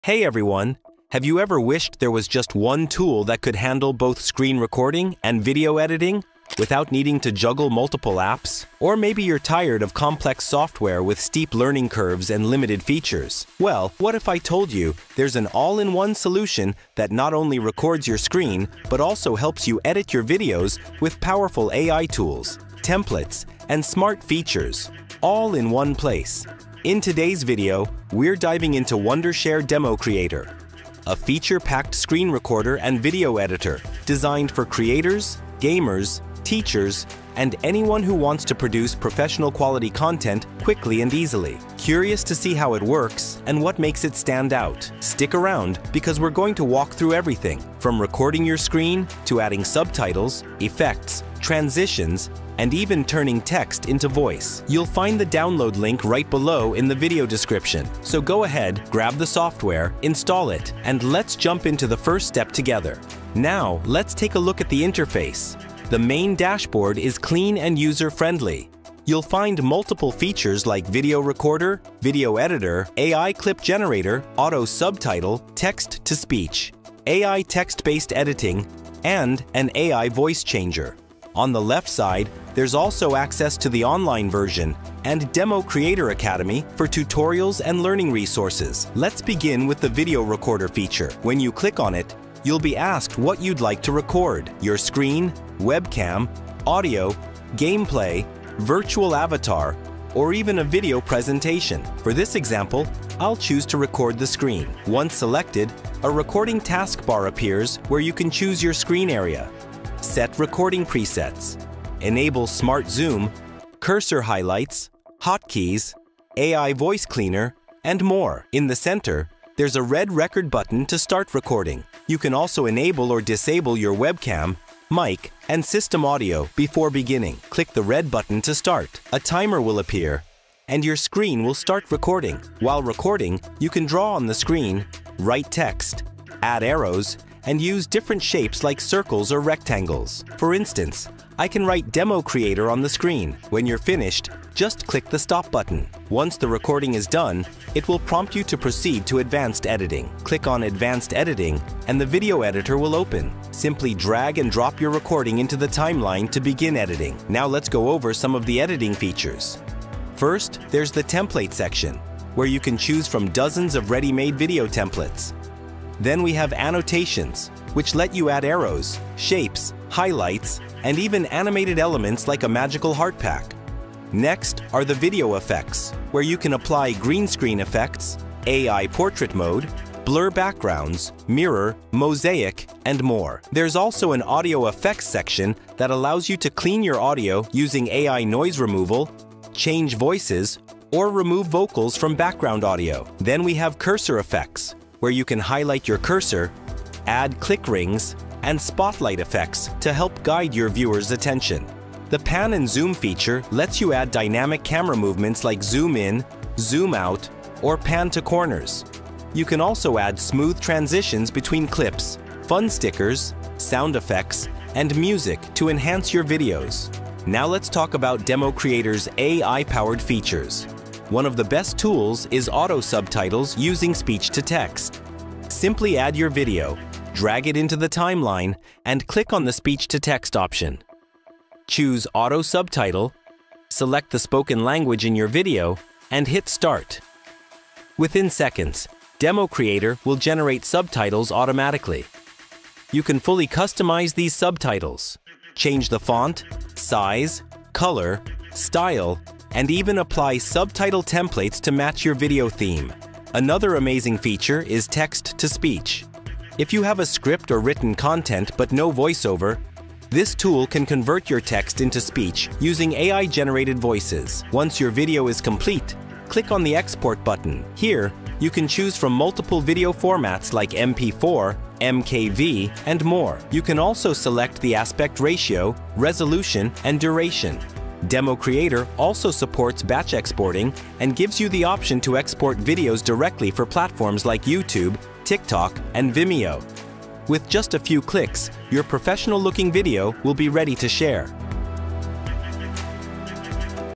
Best All-in-One Screen Recording Software & Video Editor
(Please note that this link in the video description must be above "Show more".)
**Know more about:
Wondershare DemoCreator is a versatile tool for content creators, providing seamless screen recording and video presentations.
The latest version, DemoCreator V8.8, is now live and offers an array of innovative features. It supports popular virtual cameras and includes creative visual and audio effects such as cursor effects, AI text editing, AI music, and teleprompter.
Additionally, it offers live streaming and professional templates specifically designed for educators and gamers to record and edit highlight clips.